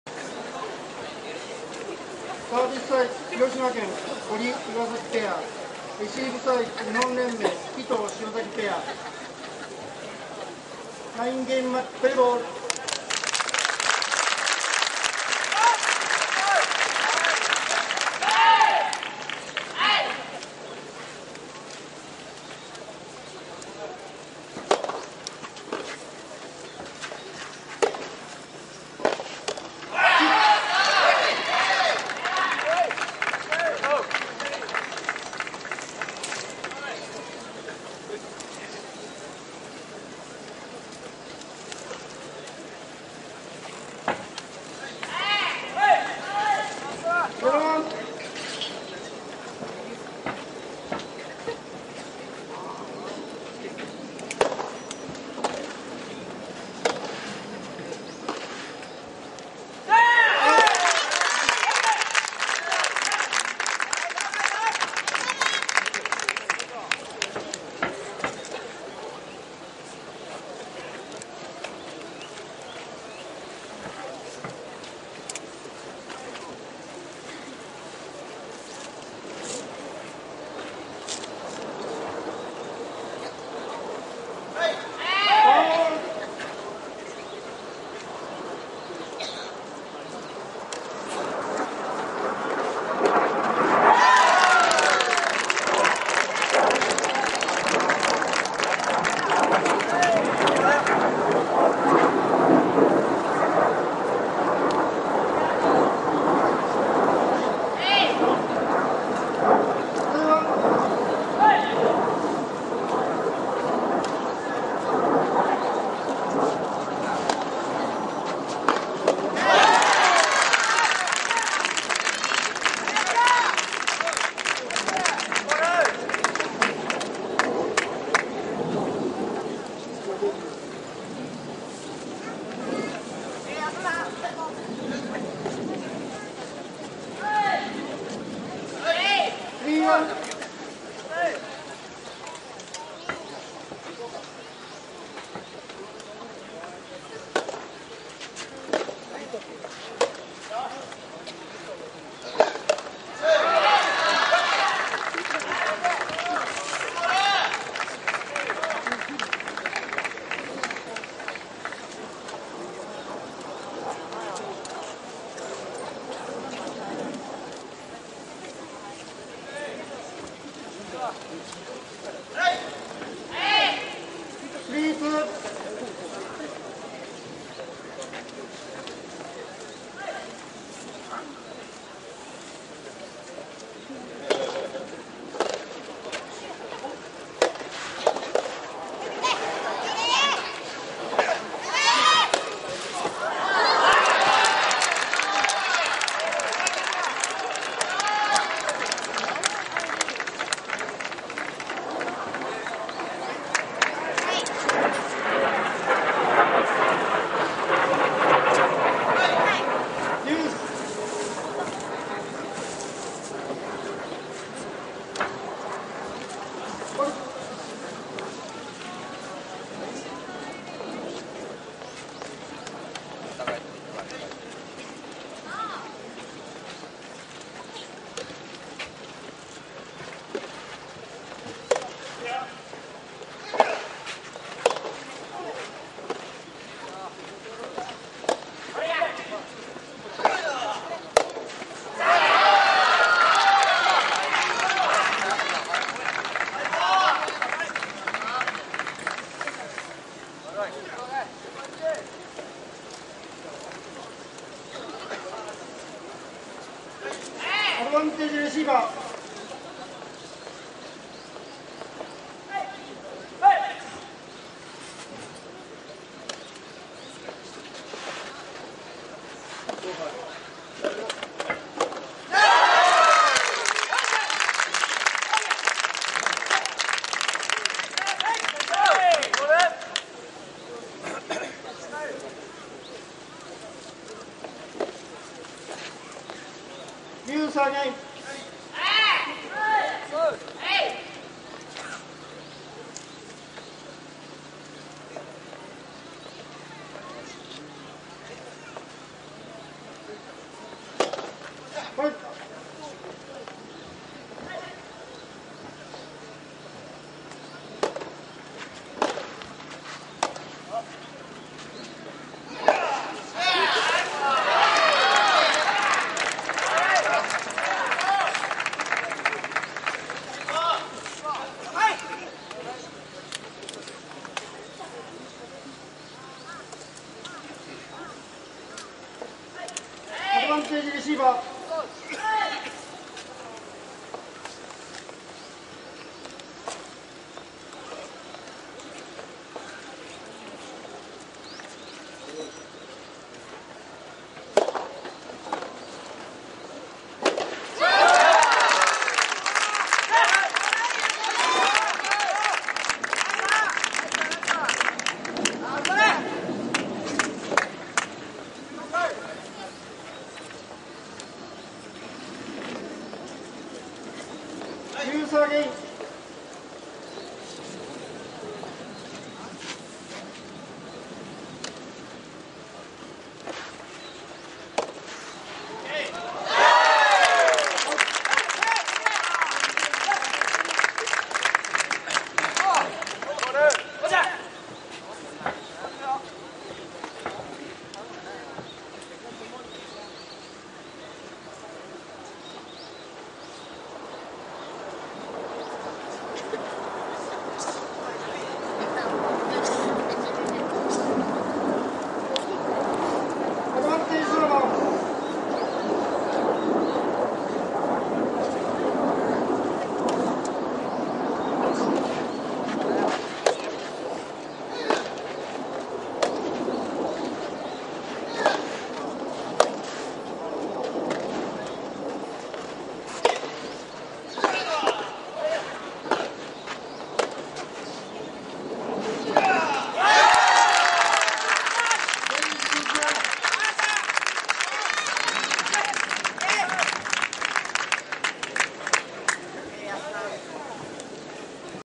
第62回 天皇賜杯・皇后賜杯 全日本ソフトテニス選手権大会 男子準決勝2-1
堀 晃大・岩﨑圭(広島県・NTT西日本広島) 対 鬼頭貴之・塩嵜弘騎(日本連盟・早稲田大学)
第1ゲーム